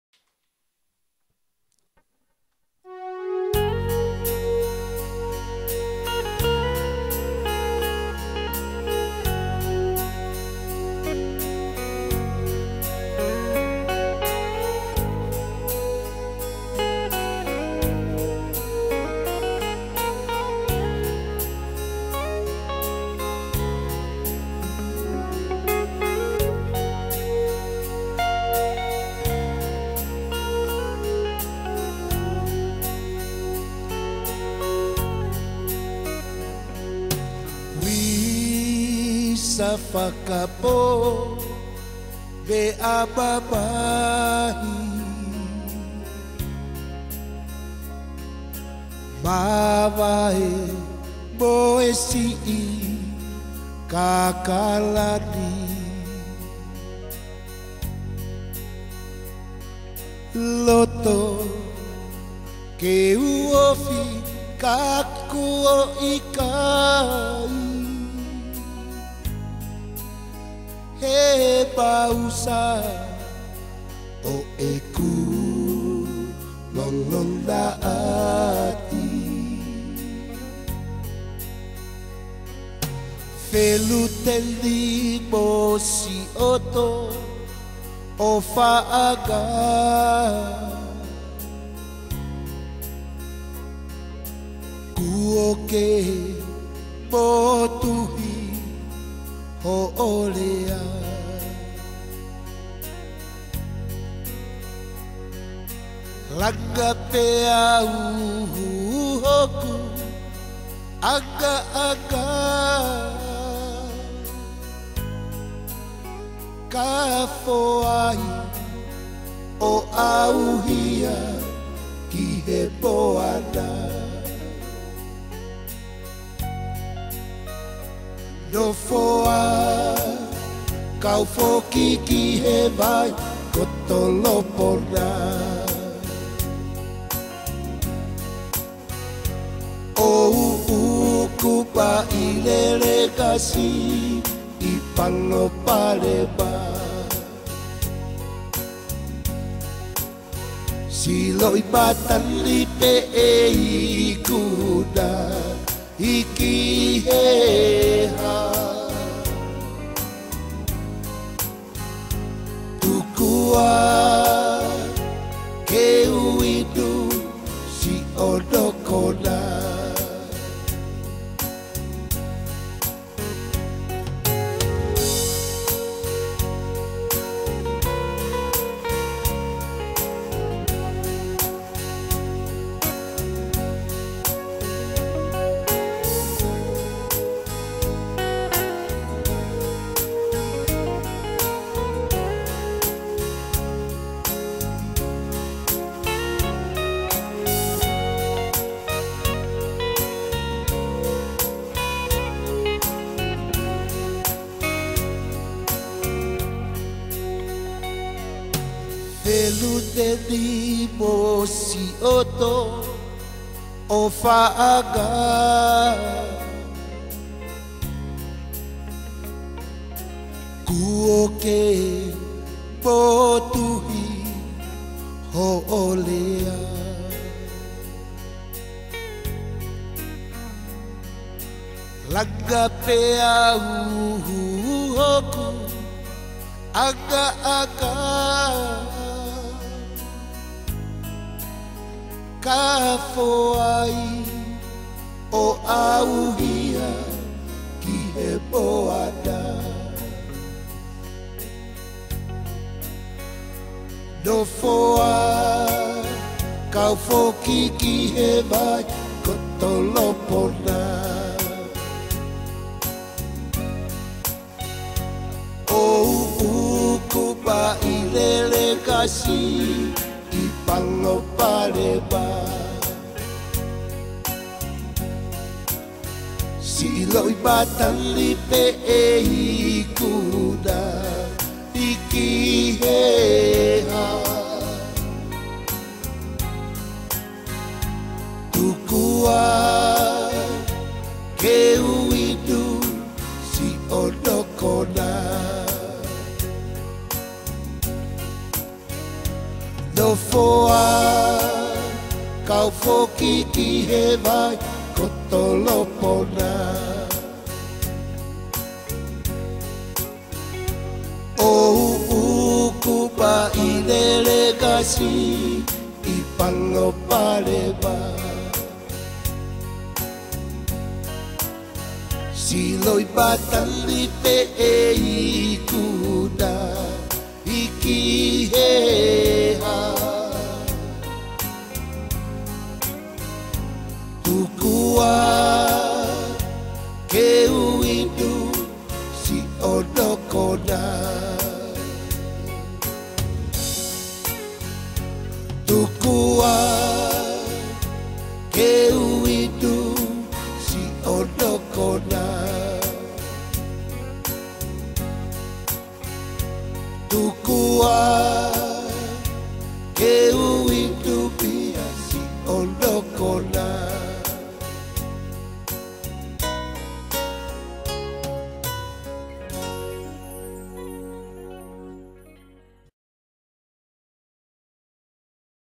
'Ulise Pole'o Tokoma'ata
Dedicate this song to my Bro. Foni Pole'o (RIP)
Composed by: Foni Pole'o Performed by: 'Ulise Pole'o Tokoma'ata